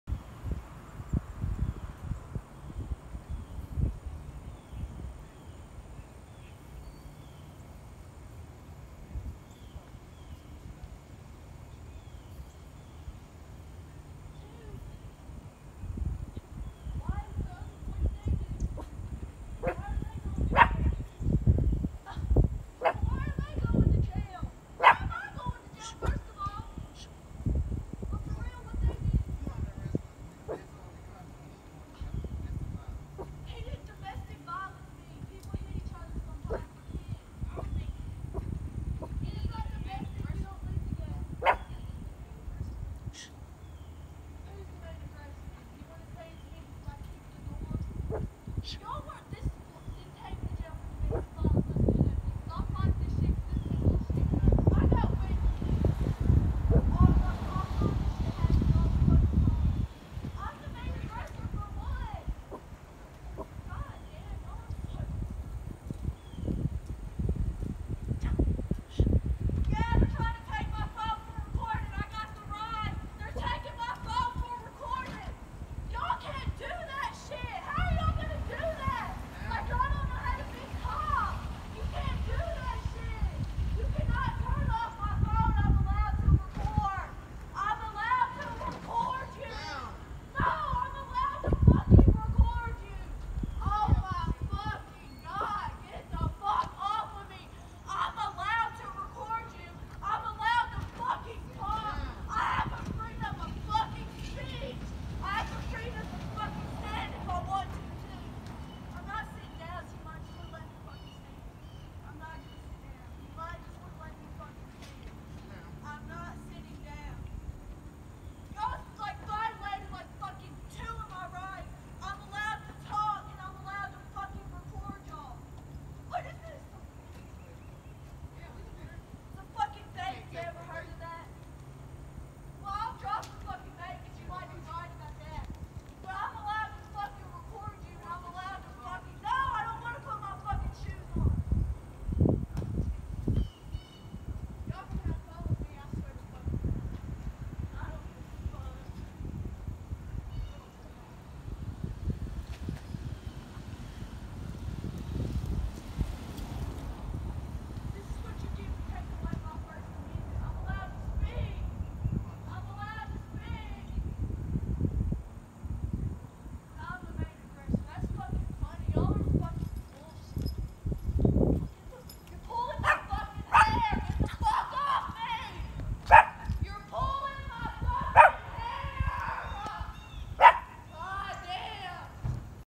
Neighbor lady gets arrested
The normally quiet neighborhood erupts at 3:00pm when a young woman across the street gets arrested for domestic violence.
Innocent until proven guilty.